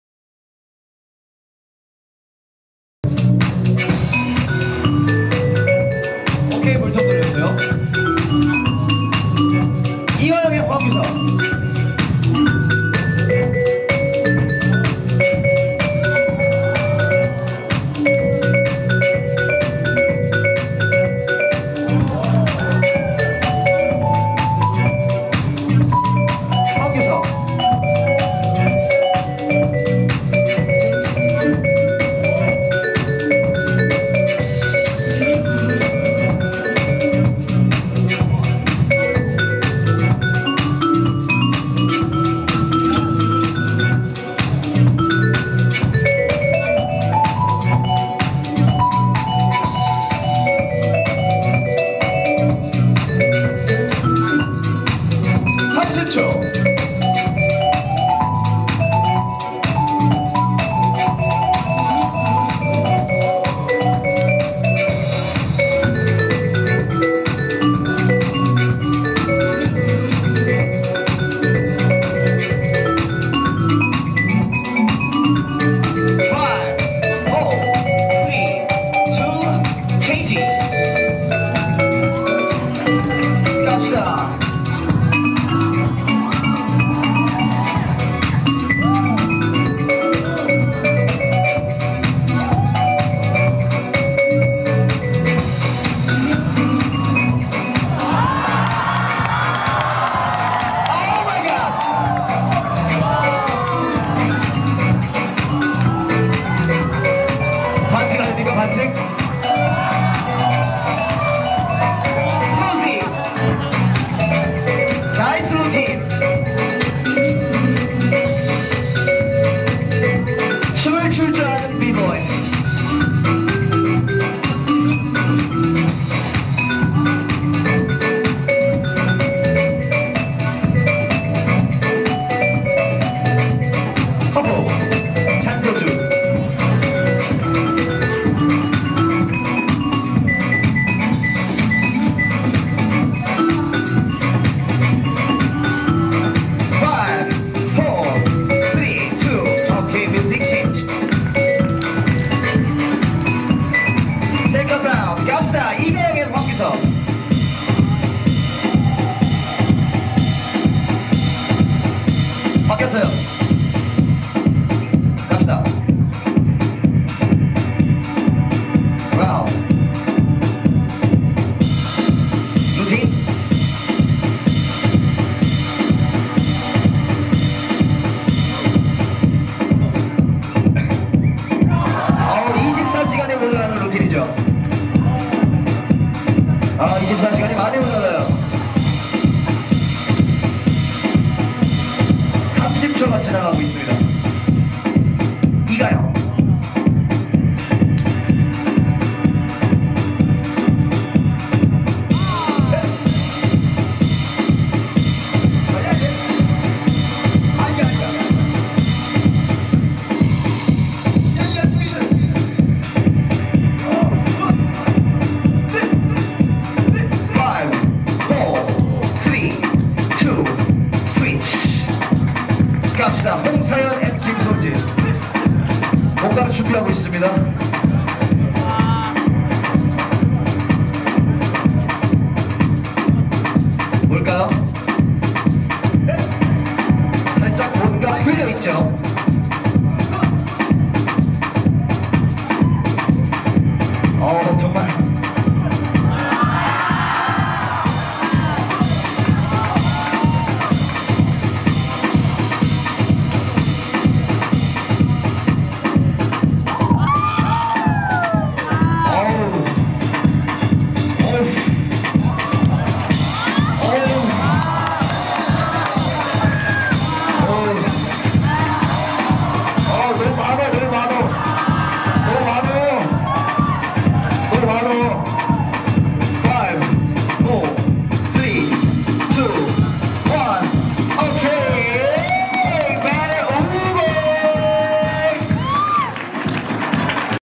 황규석, 이가영 vs 김솔지, 홍사현 / Waackin SF_2 / Dongbang Battle Vol.12 / Allthatbreak.com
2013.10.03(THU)
Dongbang Battle Vol.12 @ Kukmin Univ.

Present by BUSTA (Kukmin Univ. Dance Team)
All-Style Battle Category
Krump, Bboy, House, Waackin, Poppin, Lockin, Hiphop

[MC]
DU LOCK (Funky Street Princez)

[DJ]
AKIN (Special Guest)

[JUDGES]
BIG-J (Monster Woo Fam / Krump)
G-HAKSU (Nomad G, Rivers Crew / Hiphop)
DANDY (Winners Crew / Poppin)
WOOYON (House Of Ninja / House, Waackin)
U-BONG (Funky Street Princez / Lockin)
SHORTY FORCE (One Way Crew / Bboy)

[GUEST SHOW]
MIGHTY LOCKIDZ (Lockin)
FRONT OF HOUSE (House)
TEAM BLACKOUT (Krump)